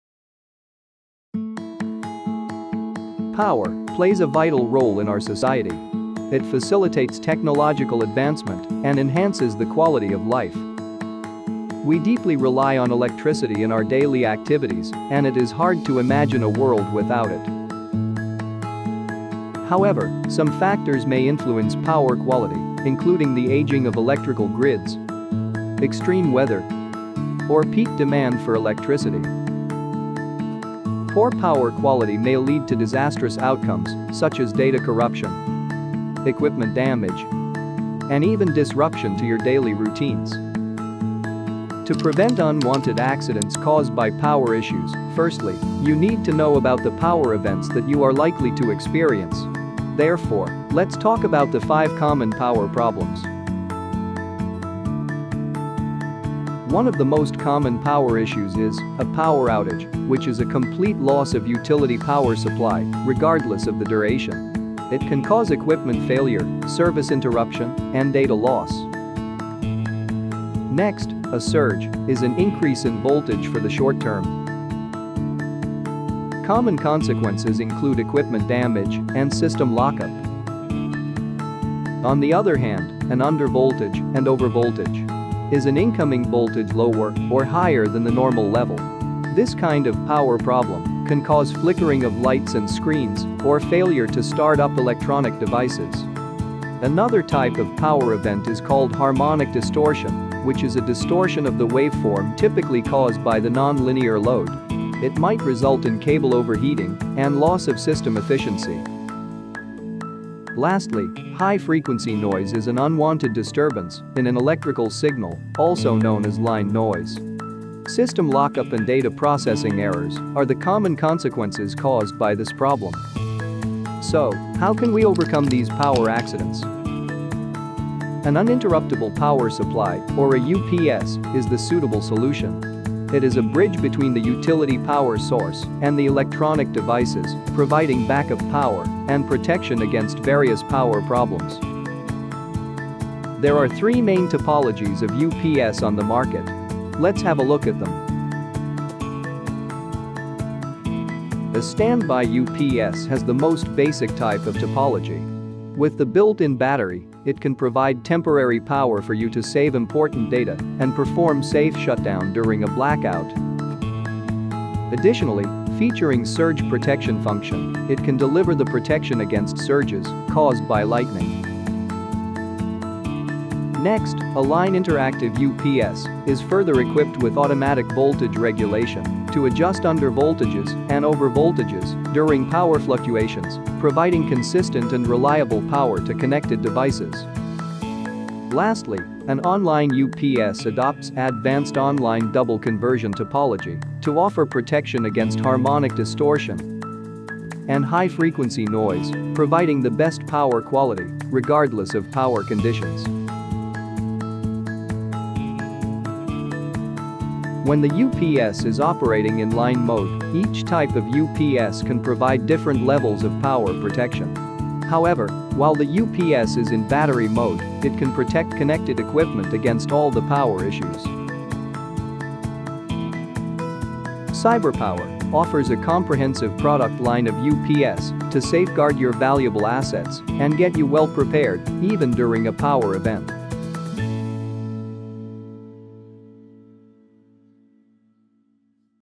Top 5 Common Power Issues
Power is a necessity in our daily lives, which brings convenience and improves human welfare. However, poor power quality can cause problems to critical devices, data and daily operations. This video describes the common power issues that you may experience and which type of UPS system can be selected to meet the level of protection needed, keeping you staying powered and protected.